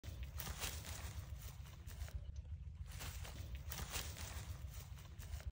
The bird was caught by Shikra hawk
Hunting with Shikra hawk hawk. She got only one.